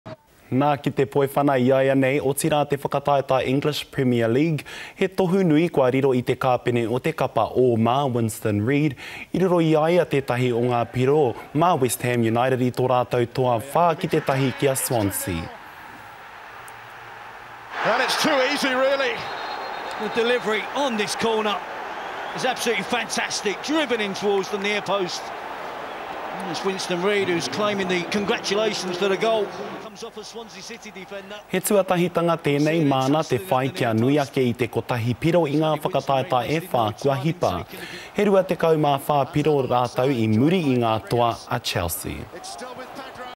Winston Reid scores big in English Premier League
To soccer now and to the English Premier League. All Whites captain Winston Reid has scored big.
He scored for West Ham United and snagged their fourth win against Swansea.
This is a first for them to have more than one point in the last four matches.

They are 24 points behind Chelsea on the premier league leader board.